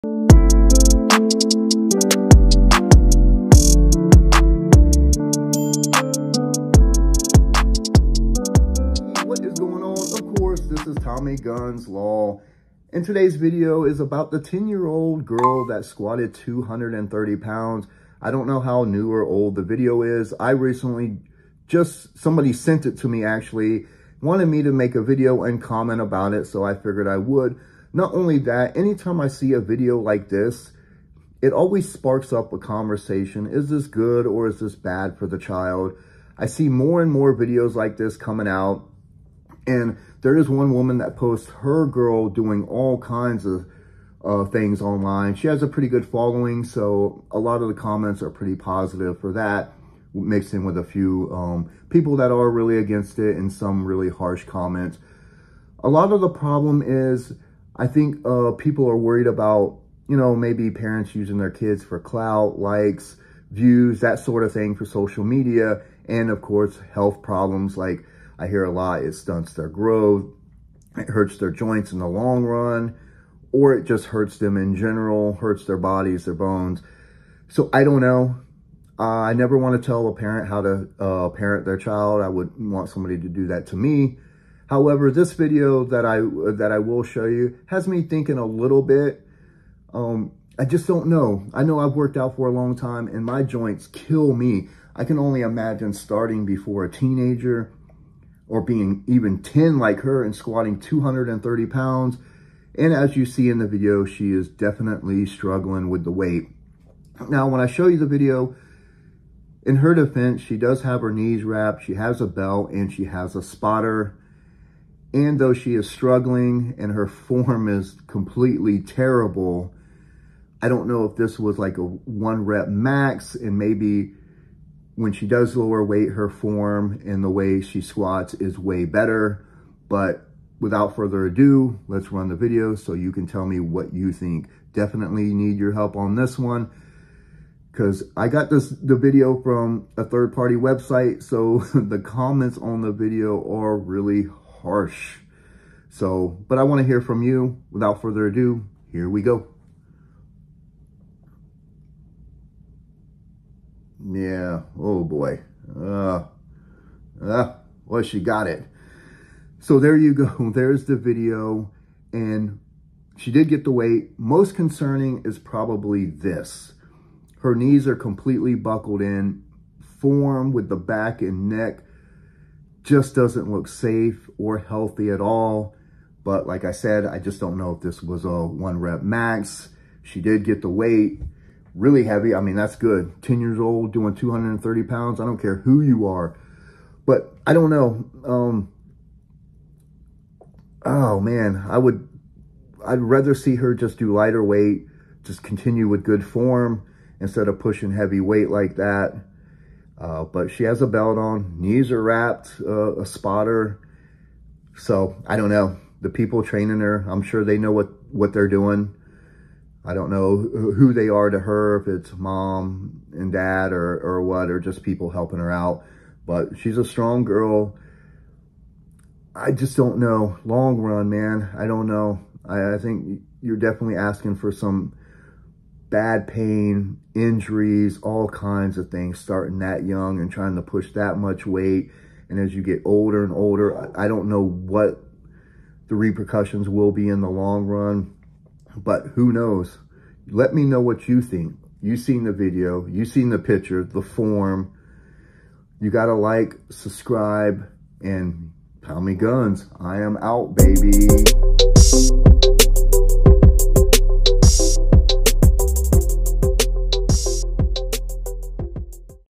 10 Yr. Old Girl Squats 230lbs!! This amazing, or bad? My thoughts.
10 year old girl squats 230lbs!! Some would say this is amazing and others not so much. Videos like this always spark conversation. ￼Some really think this is bad on children and will hurt them in the long run forsure. Where others have no problem with it at all . ￼ I give you my thoughts, and I’m looking for yours .